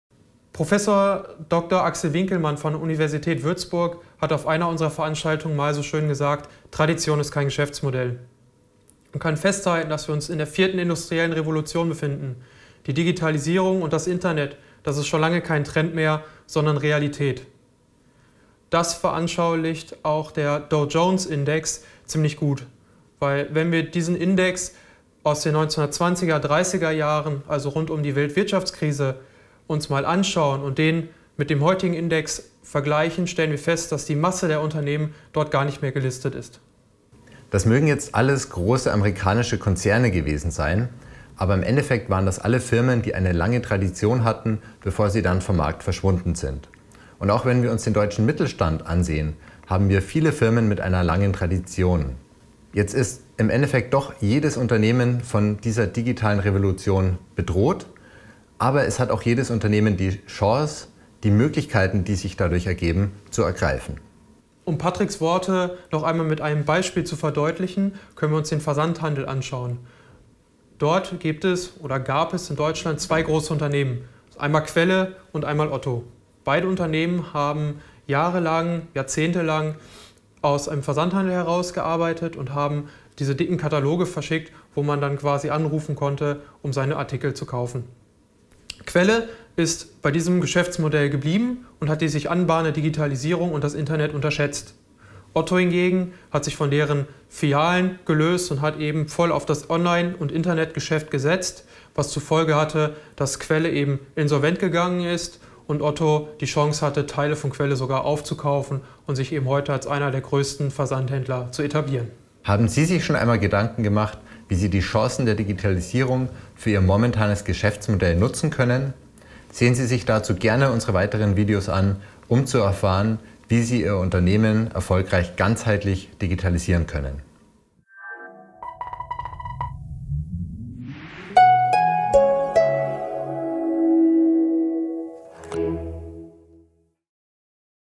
VLOG: Tradition ist kein Geschäftsmodell | DigiCheck Strategy Videoserie
Prof. Dr. Axel Winkelmann auf diesen Satz auf einer unserer Kundenveranstaltungen gesagt. Diese Aussage trifft den Nagel auf den Kopf. Jede Industrie, jede Branche, jedes Unternehmen (egal wie groß) muss sich mit der digitalen Transformation beschäftigen. Es gibt genügend internationale und deutsche Beispiele von eigentlich starken Unternehmen, die das Internet und die aufkommende Digitalisierung unterschätzt haben.

Erfahren Sie in dieser Videoserie, wie sie Ihr Unternehmen erfolgreich durch die digitale Transformation führen können: